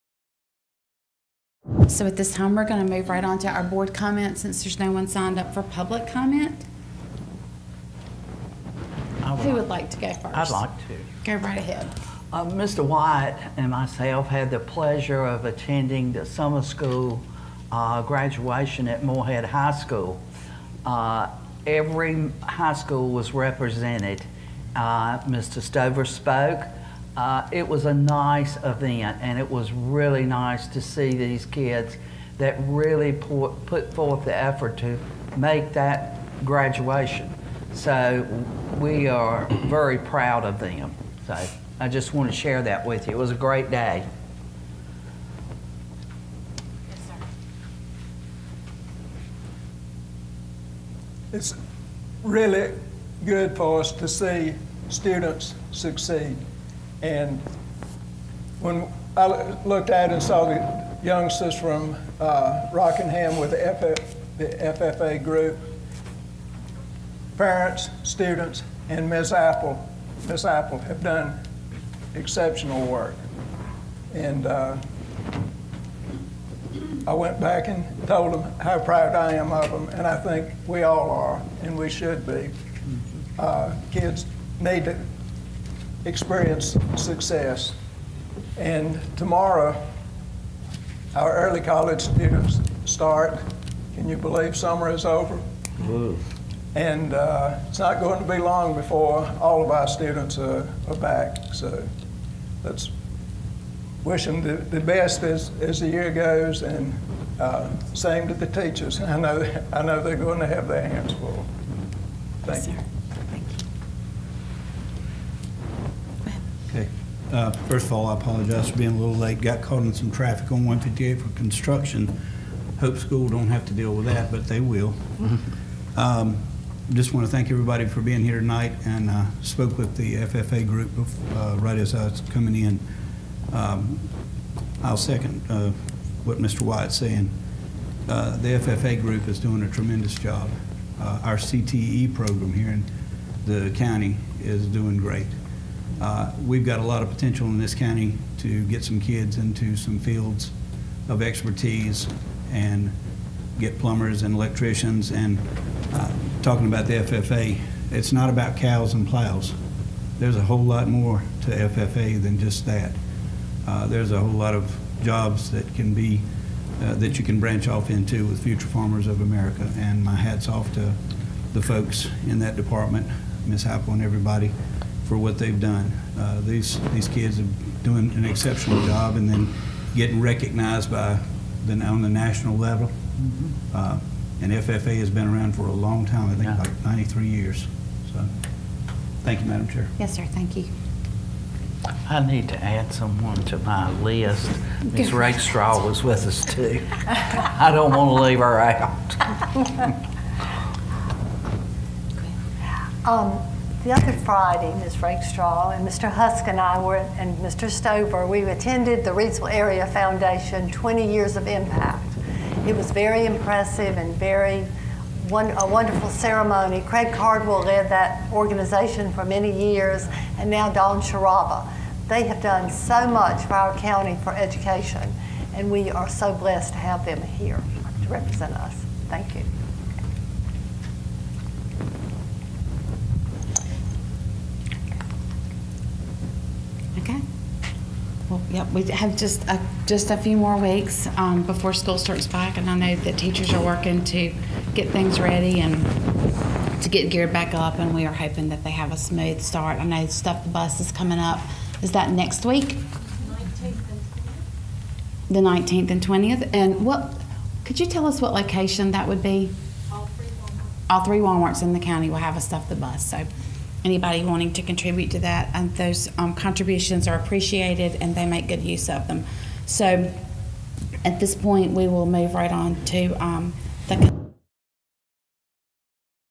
Board Member Comments - August 8, 2022 - Rockingham County Board of Education Meeting
Video Clip of the August 8, 2022 Rockingham County Board of Education Meeting Agenda Item 5.2 Board Comments